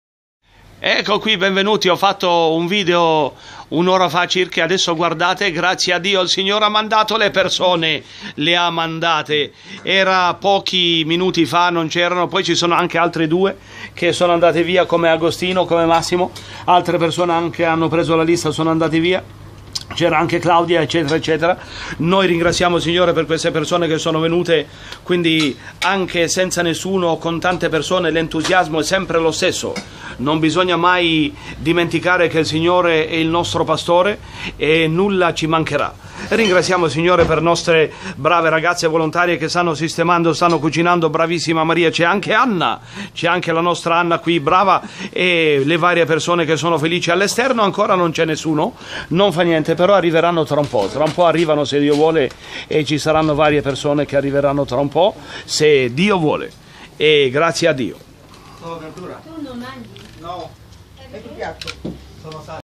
Anna, Samba, Delia ecc dopo ZERO persone sono venu
E grazie a DIO sono venute le persone come al solito  ... il ritardo era dovuto all'incidente nella metropolitana.